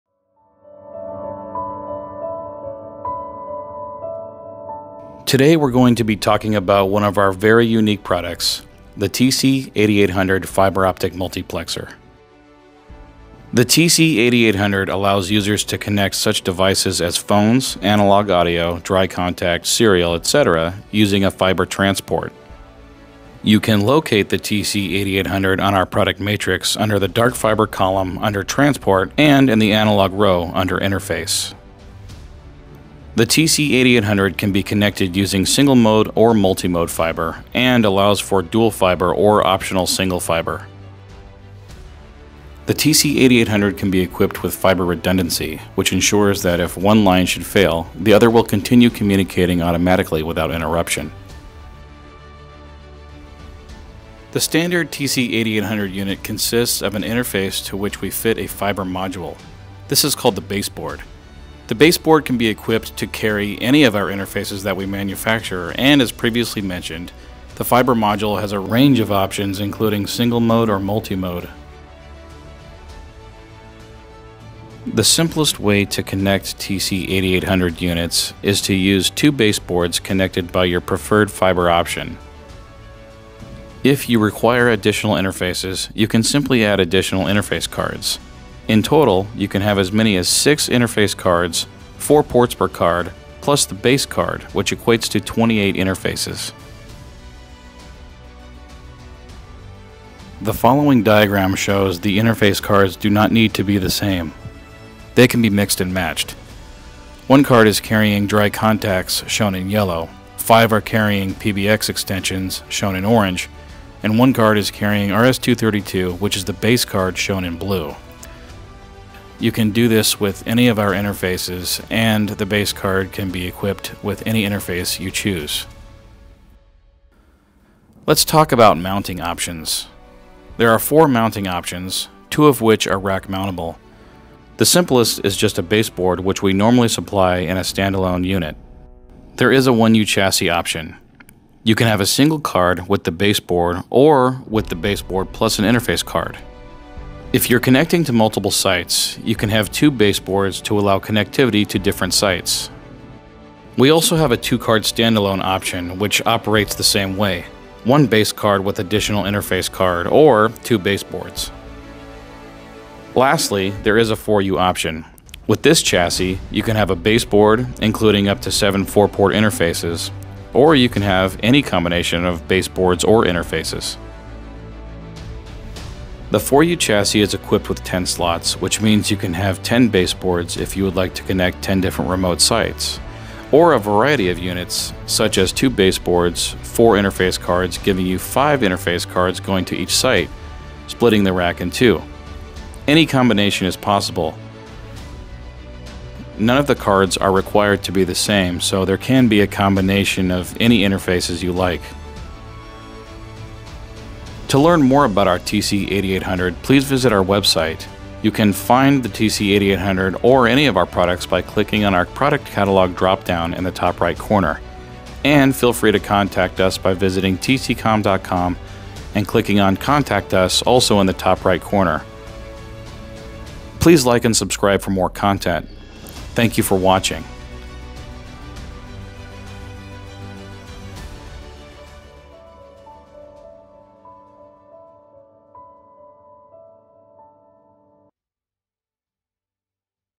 Easy-to-implement fiber optic multiplexer | TC8800
Multiplexing continues to be a huge cost saving and easy-to-implement networking solution. Our fiber optic multiplexers connect a single channel to one of two or four channels. Mix and match telephone, data & analog channels to remote network sites. The TC8800 can extend up to 28 channels and is hot swappable along with high temperature and extreme temperature options.

Use the Product Matrix to find the right TC Communications product for your application